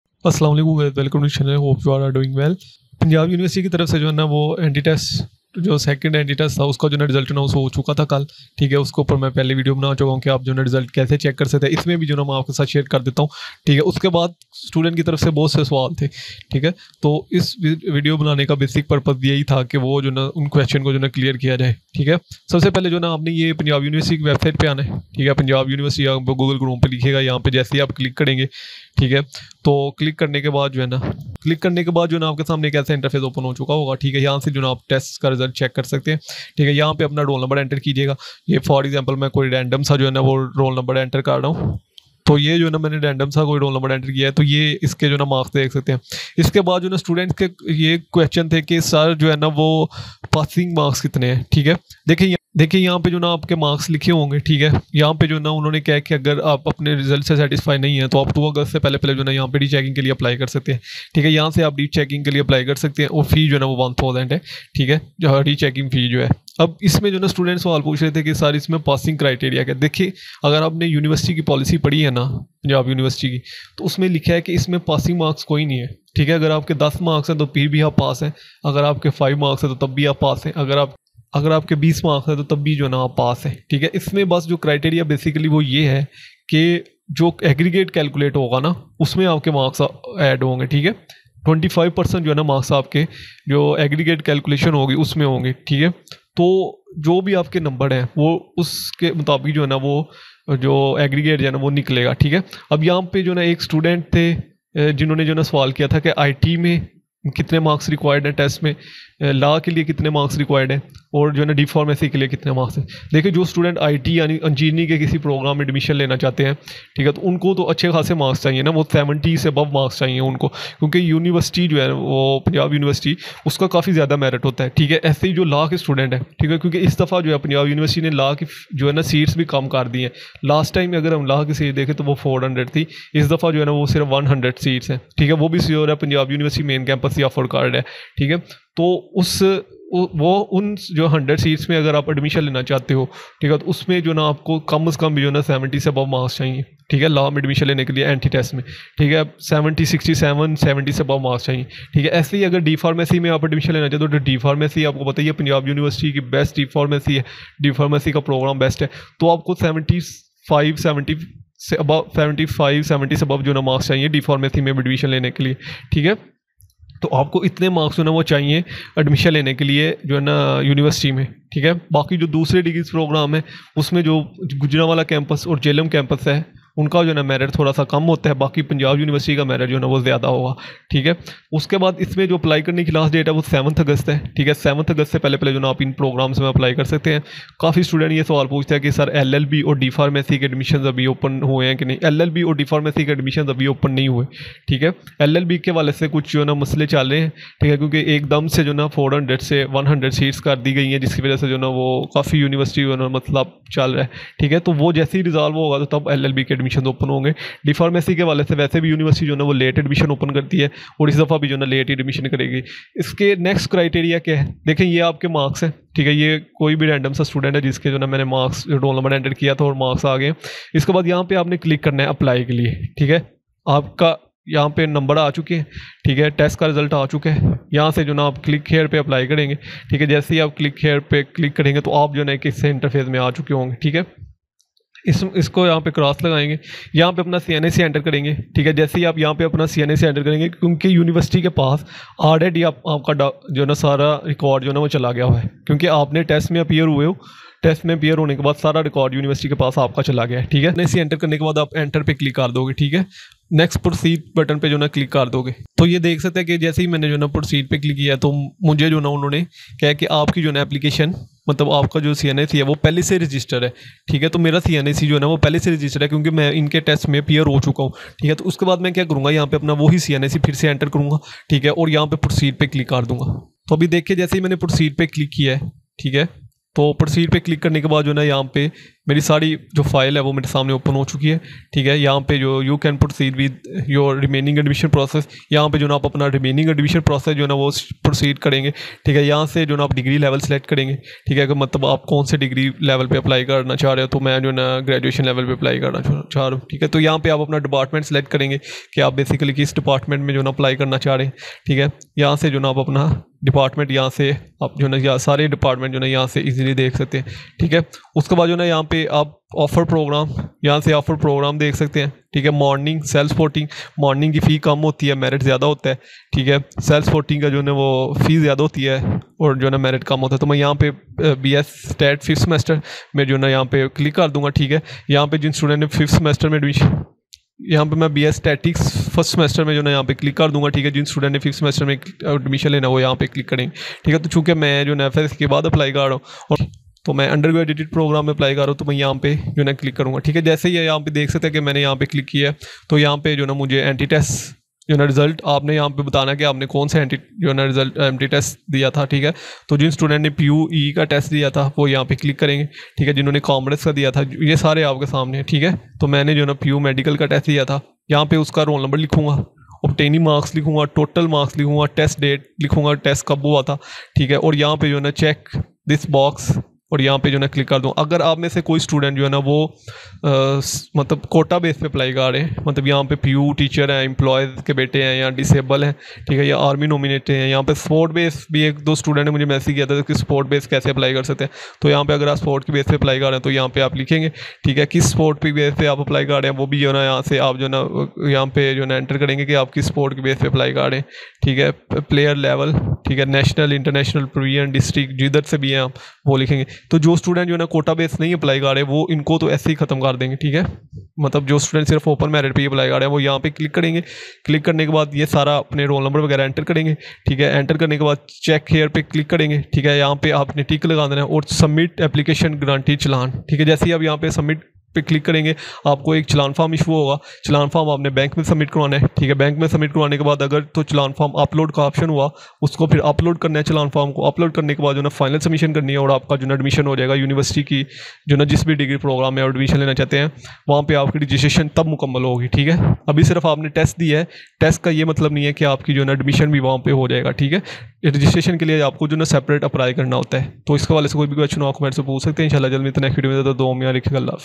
PUNJAB UNIVERSITY 2nd ENTRY TEST RESULT 2023 :: what to do Next? | Passing Marks & Merit calculation
Punjab University has announced the result of the PU 2nd Admission Test 2023. University of Punjab (PU), Lahore organized the first entry test on Sunday, June 25, 2023, for admissions to its undergraduate programs. More than 41 thousand students registered for the PU Entry Test 2023.

Examination centers were established for the entrance test in Lahore, Karachi, Quetta, Peshawar, Gilgit, Skardu, Dera Ismail Khan, and other important cities of the country. 141 examination centers were established across Pakistan while 88 centers were established in Lahore. 7 types of entry tests are being conducted for admissions in various undergraduate programs.
PU 2nd Admission Test Result 2023
The result of the PU 2nd entry test is also announced. Students can check their results by entering their roll number